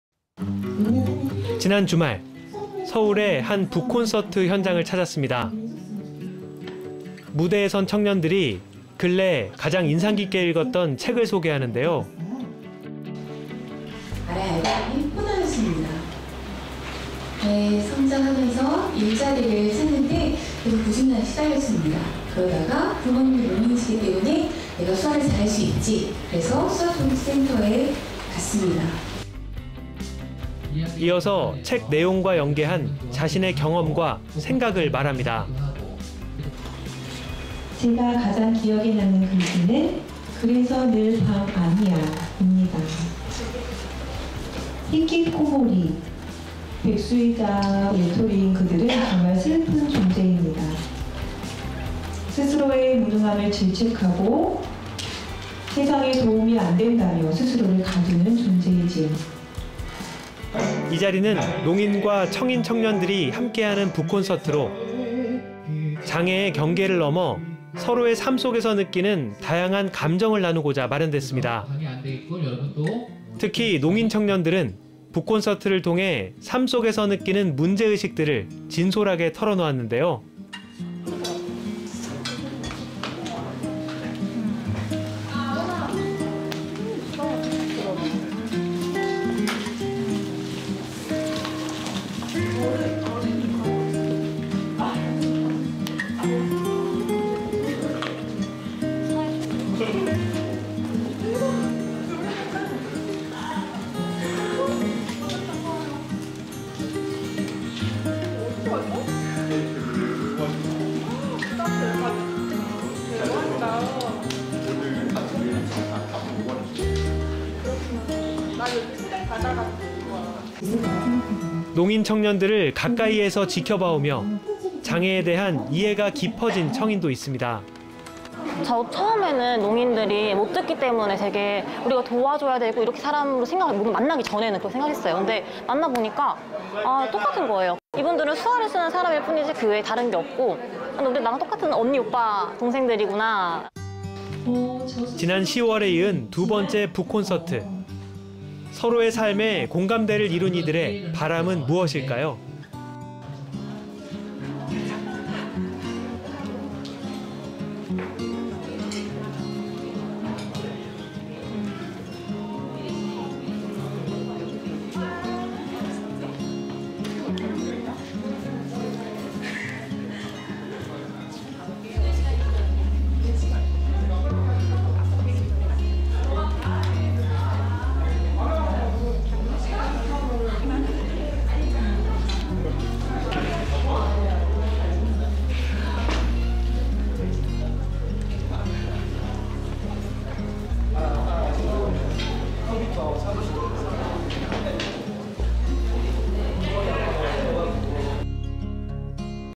2020.01.01 [교육현장 속으로] 농인과 청인 청년들의 북콘서트 '어쩌다 청춘'
지난 주말, 서울의 한 북 콘서트 현장을 찾았습니다.
무대에 선 청년들이 근래 자신이 가장 인상 깊게 읽었던 책들을 소개하는데요,

“(주인공인) 아라이는 코다였습니다. 성장하면서 일자리를 찾는데 구직난에 시달렸습니다. 그러다가 ‘부모님이 농인이시기 때문에 내가 수화를 잘 할 수 있지’ 그래서 수화통역센터에 갔습니다.”

이어 책 내용과 연계한 자신의 경험과 생각을 말합니다.

“제가 가장 기억에 남는 글귀는 ‘그래서 늘 방 안이야’입니다. 히키코모리. 백수이자 외톨이인 그들은 정말 슬픈 존재입니다. 스스로의 무능함을 질책하고 세상에 도움이 안 된다며 스스로를 가두는 존재이지요.”

이 자리는 농인과 청인 청년들이 함께 하는 북콘서트로 장애의 경계를 넘어 서로의 삶 속에서 느끼는 다양한 감정을 나누고자 마련되었습니다.
특히 농인 청년들은 북콘서트를 통해 삶 속에서 느끼는 문제의식들을 진솔하게 털어놓았는데요,

인터뷰: 조예림 / 간호조무사
“저는 이제 막 사회에 나와 1년, 이제 2년이 되었기 때문에 경험이 너무 부족해서 사람들을 대할 때 쉽지 않은 점이 있습니다. 가장 보람 느낄 때 친절하게 서비스를 드린 후 끝나고 가시면서 도와주셔서 너무 감사하다고 인사를 해주시면 보람을 많이 느낍니다.”

인터뷰: 신태완 / 회사원
“농정체성이 없다면 청인과 같이 생각하고 길을 잃어버리게 돼요. 농정체성에 눈을 떠야 내 길을 개척할 수 있고 농사회에서 청인과 동등하게 평등한 생각을 가질 수 있습니다.”

농인 청년들을 가까이에서 지켜봐 오며 장애에 대한 이해가 깊어진 청인도 있습니다.

인터뷰: 채주연 / 수어통역사
“처음에는 농인들이 못 듣기 때문에 우리가 도와줘야 되고 만나기 전엔 이렇게 생각했어요. 근데 만나보니까 그냥 똑같은 사람인 거예요. 이분들은 수화를 쓰는 사람들이지 다른 건 없고 나랑 똑같은 언니, 오빠, 동생들이구나.”

지난 10월에 이은 두 번째 북 콘서트.
서로의 삶에 공감대를 이룬 이들의 바람은 무엇일까요.

인터뷰: 유승민 / 서울청각장애인학습지원센터 학습지원팀
“1회 때 북콘서트 내용을 보면 농인과 청인이 함께 한 것이 맞는데, 제 입장에서 보면 의사소통(매체)이 중요한 것이 아니라 서로의 삶에 대해 공유하는 것이 가장 중요하다고 생각합니다.  청인의 입장에선 농인의 삶이 장애인의 삶과 같다고 생각하는 것이 당연한데, 이 행사를 통해 농인의 진짜 삶을 자세히 들여다보면 자연스럽게 '농인의 삶이 정말 힘들었겠구나'를 깨닫게 됩니다.”

인터뷰: 이샛별 / 회사원
“오늘 행사를 통해서 각각 농인의 삶과 상황에 대해서 여러분들이 이해하고 공감하시며 이 시간을 통해 농인을 만날 때 이렇구나 하고 공감해 주시는 여러분이 되시길 바랍니다.”